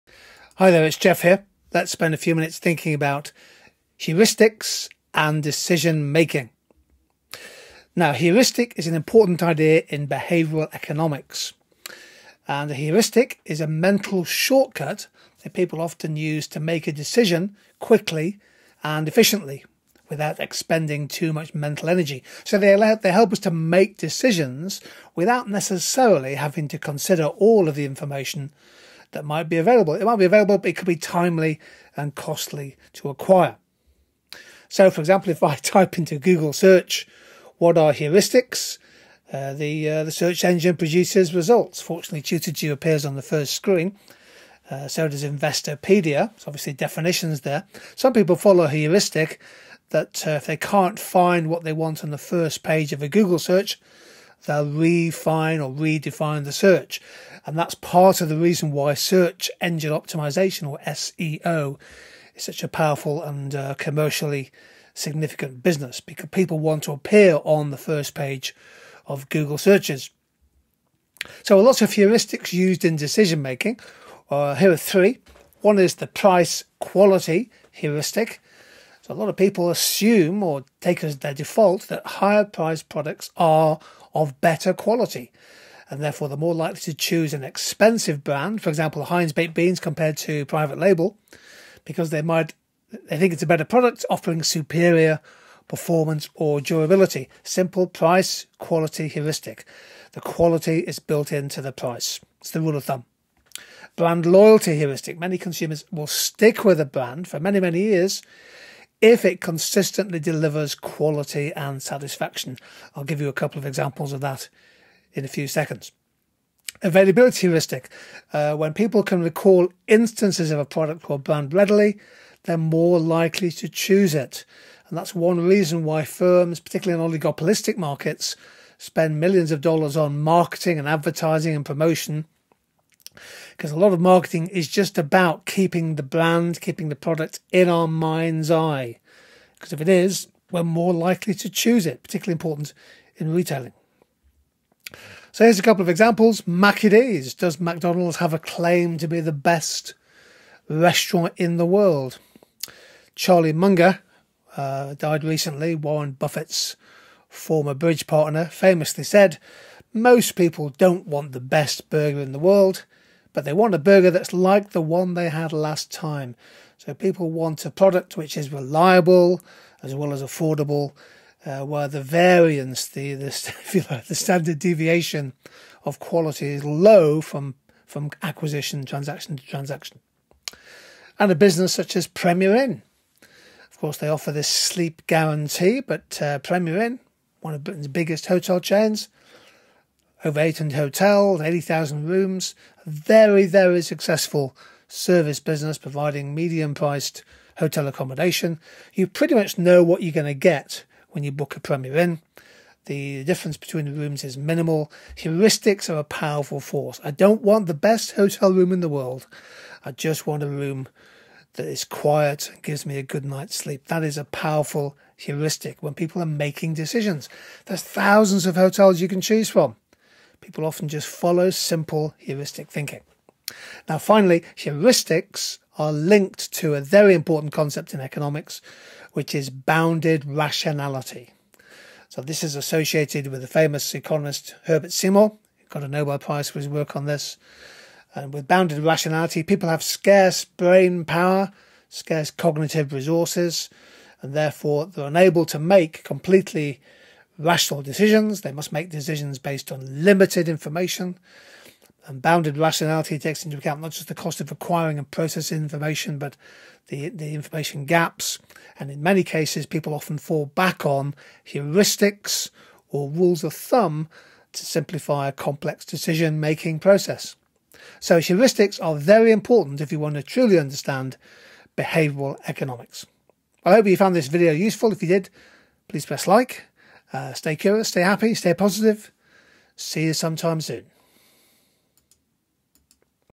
Behavioural Economics - Heuristics in Decision-Making
This revision video looks at bounded rationality and heuristics. In economics, heuristics refer to mental shortcuts or rules of thumb that people use to make decisions, particularly when facing complex or uncertain situations.